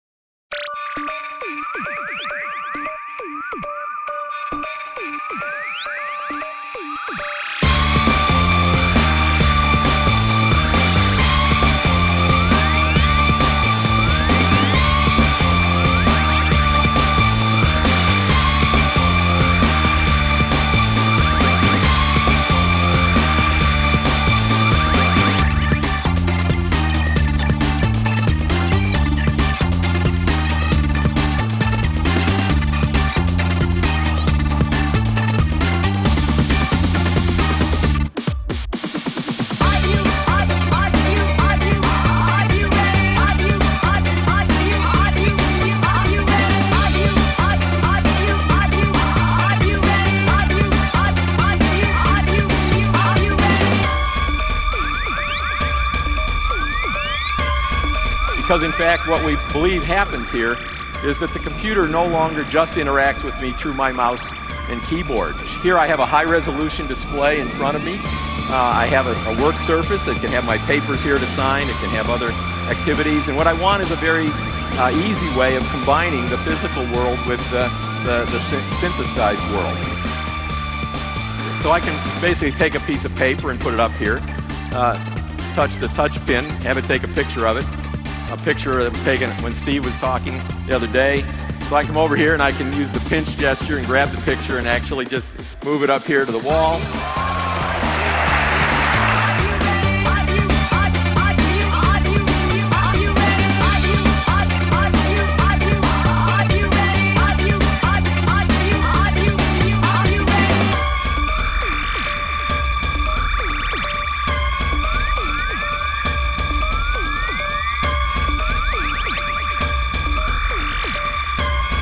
Kinect for Windows 應用集錦
Kinect 這個劃時代的體感操控技術，身為開發人員的你，可利用 Kinect for Windows SDK 搭配 Visual Studio 2010 來開發各式各樣的創意應用。
最完整的邊做邊學資源，包括Kinect開發白皮書、開發範例及學習影片，協助你對開發Kinect應用程式有更全面的了解